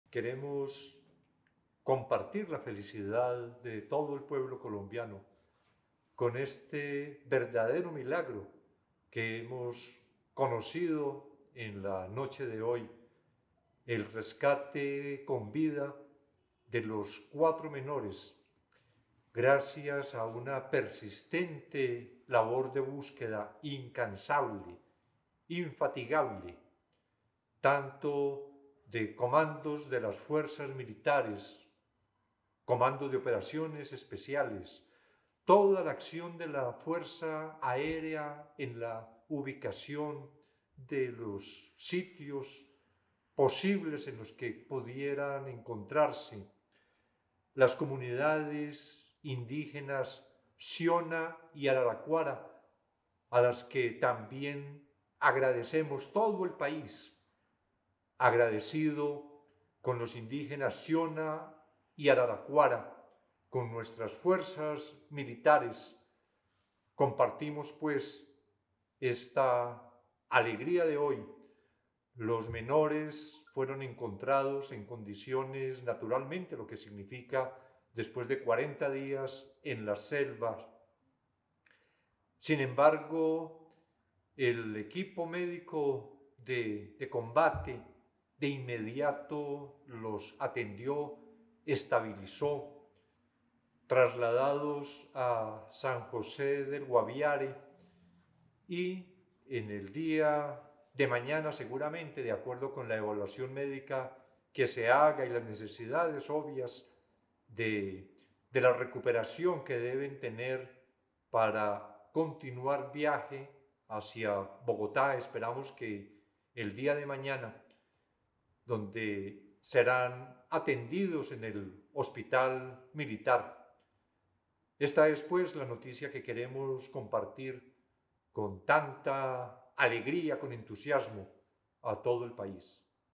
Mensaje del ministro Iván Velásquez luego de encontrar con vida a niños perdidos 40 días en la selva
El ministro Iván Velásquez destacó la labor incansable de las Fuerzas Militares y las comunidades indígenas Siona y Araracuara; anunció que los menores fueron estabilizados por el equipo médico de combate y serán trasladados el día de mañana al hospital militar.

"Compartimos la felicidad de Colombia con este milagro de encontrar a los 4 niños, gracias a la persistente labor  de búsqueda incansable e infatigable de los comandos de las Fuerza Militares"